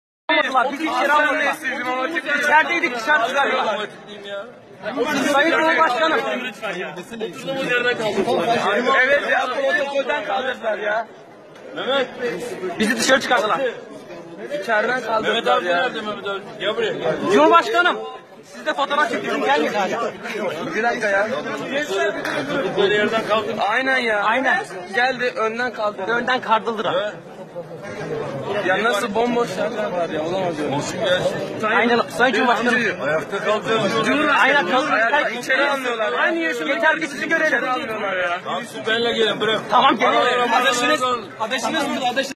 Recep Tayyip Erdoğan: Siz benle gelin, aralara maralara dalın :)
Cumhurbaşkanı Recep Tayyip Erdoğan, İçeriye alınmayan çocuklara Siz "benle gelin, aralara maralara dalın." diyor :))